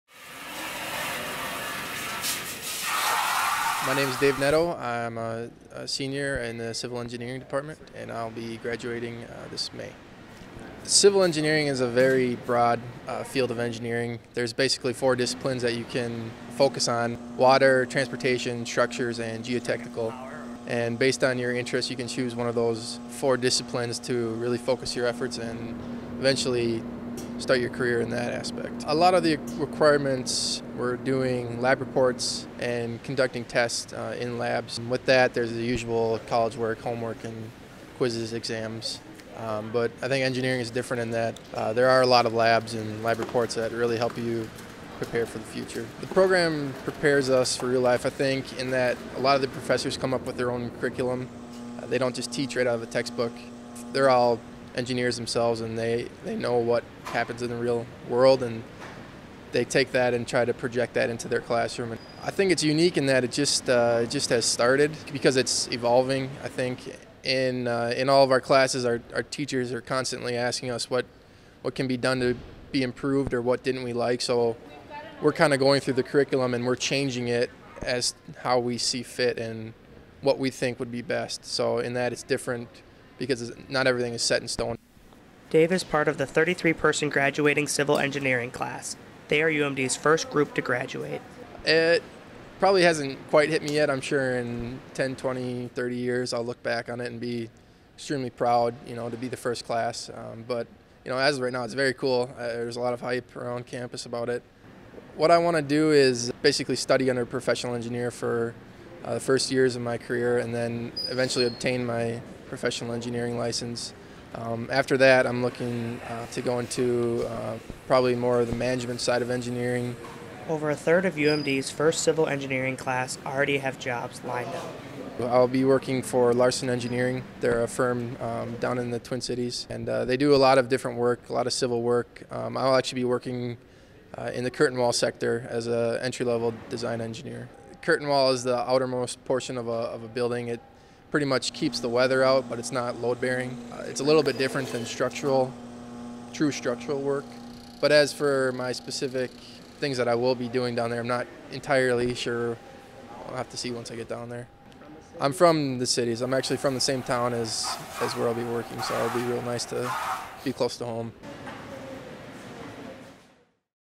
Dave Nadeau Civil Engineering Graduate
This is a video about graduating senior Dave Nadeau. In the video Dave talks about UMD's Civil Engineering Program and how he is part of the first graduating class.